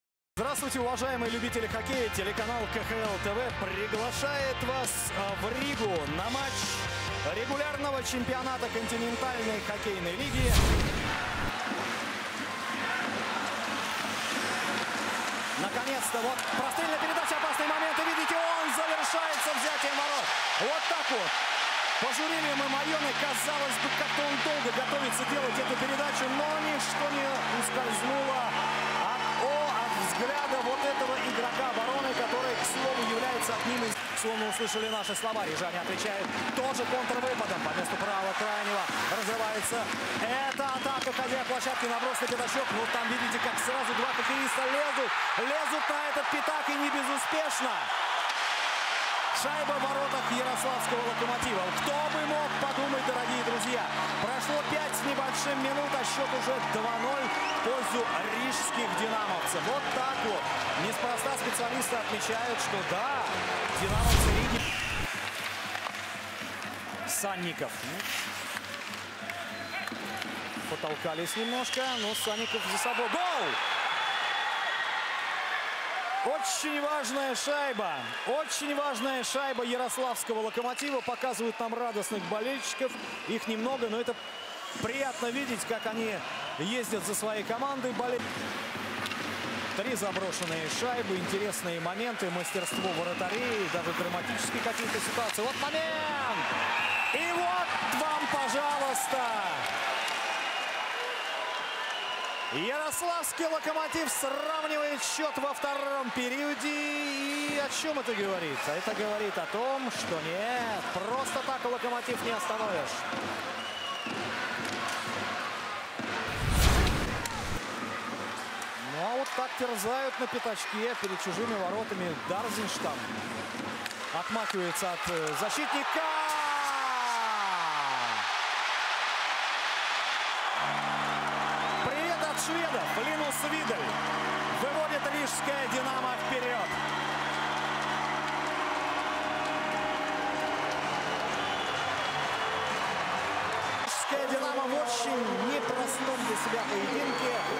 Lokomotiv 2 Dinamo R 3, 1 November 2018
1:0 - Редлихс Микелис (Дарзиньш Лаурис) 4:17 В равенстве
2:0 - Мейя Гинтс (Гимаев Сергей, Гегерис Эмилс) 5:35 В равенстве
2:1 - Аверин Егор (Аверин Николай) 30:27 В равенстве
2:2 - Рафиков Рушан (Алексеев Денис В., Картаев Владислав) 32:39 В равенстве
3:2 - Виделль Линус (Редлихс Микелис) 48:21 В равенстве

Вратари: Билялов Тимур - Коновалов Илья Г.
Броски: 20 - 39
Штраф: 6 - 6
Вбрасывания: 28 - 28